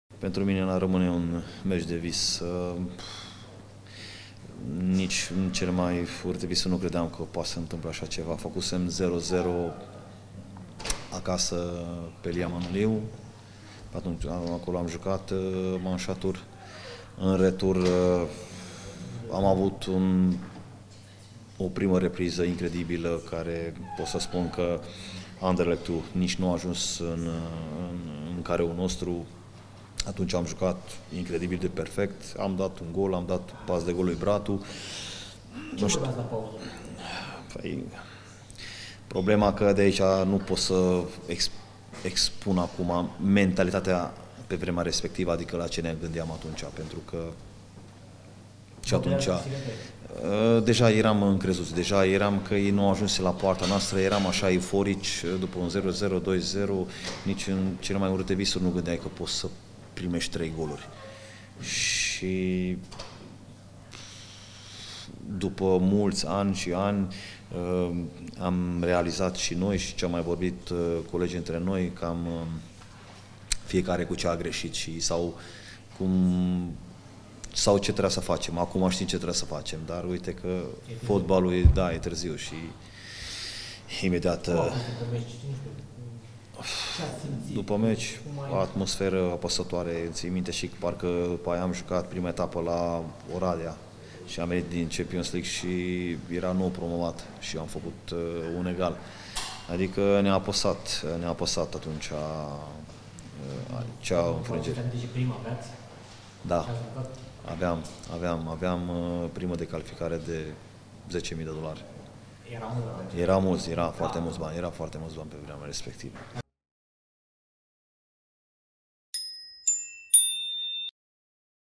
Robert Ilyeș despre meciul care încă le dă fiori rapidiștilor: „Eram prea încrezuți și euforici!”
La 47 de ani, Robert Ilyes nu e doar secundul lui Leo Grozavu la Sepsi. Mai evoluează din când în când pentru echipa a doua a clubului în Liga a 3-a, reușind chiar să înscrie în acest sezon. Într-un interviu pentru Gazeta Sporturilor, Robert Ilyes, și-a făcut radiografia carierei sale.